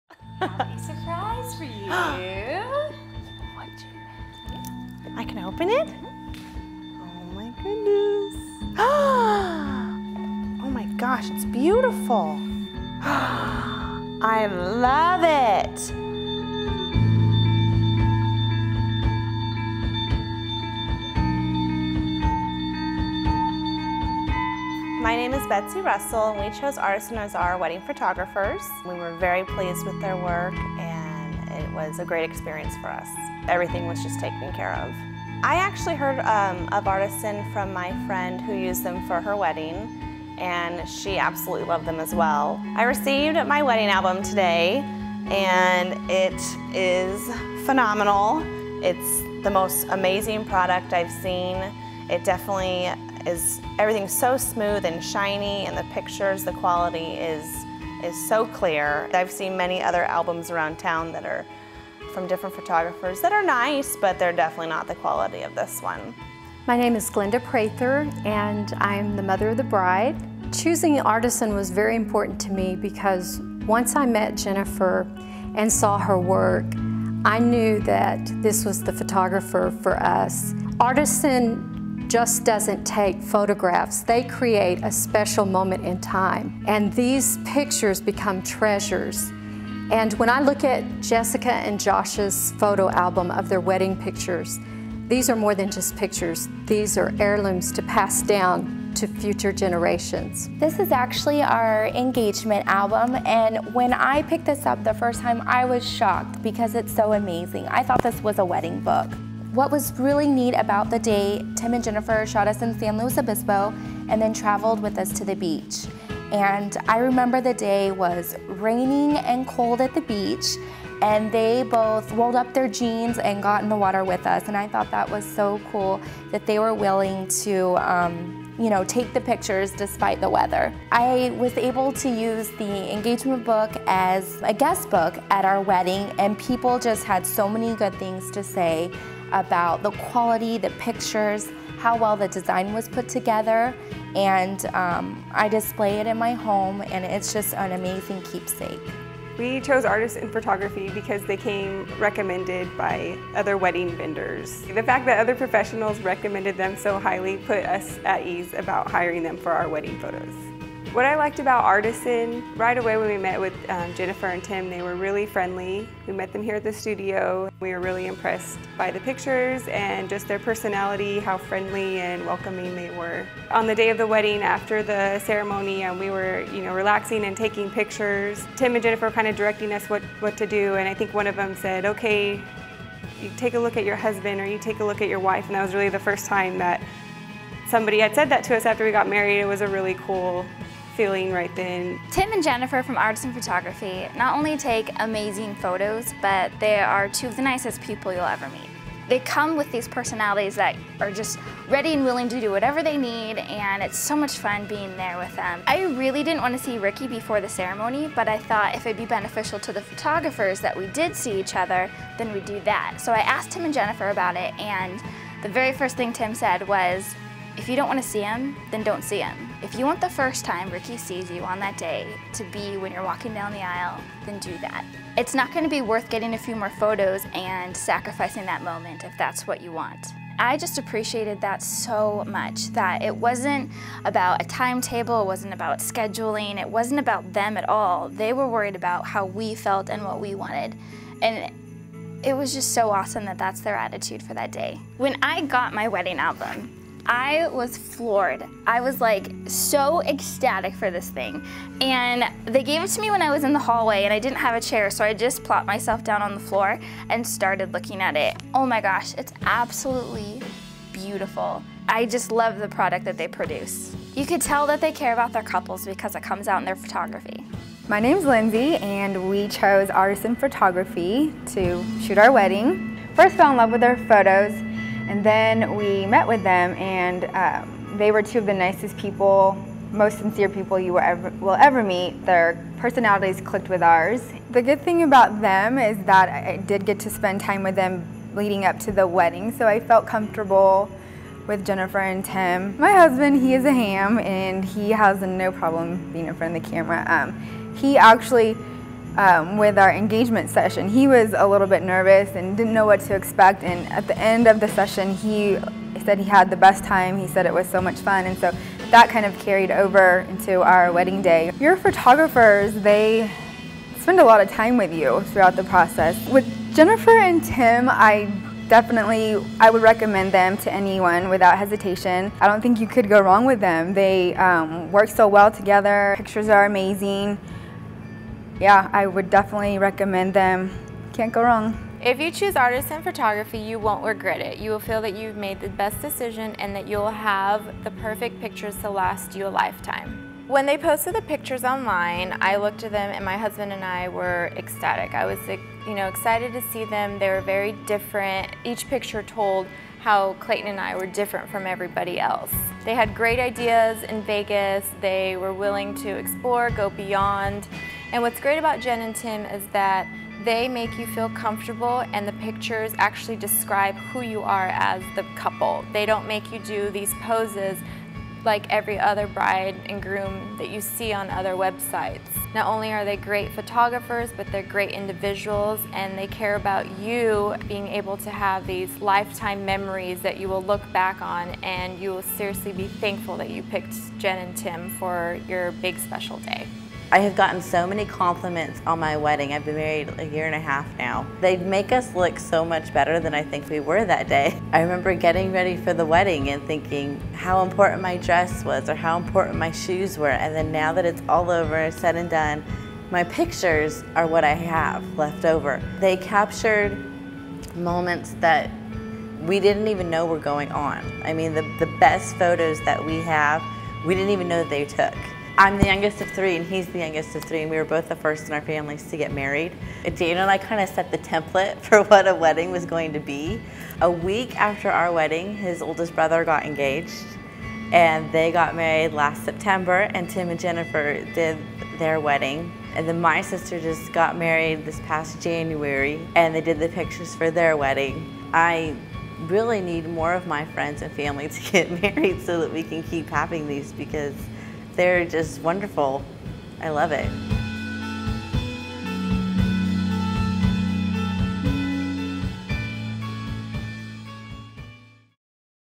Artisan Testimonals Video
Testimonials video from brides of Artisan Photography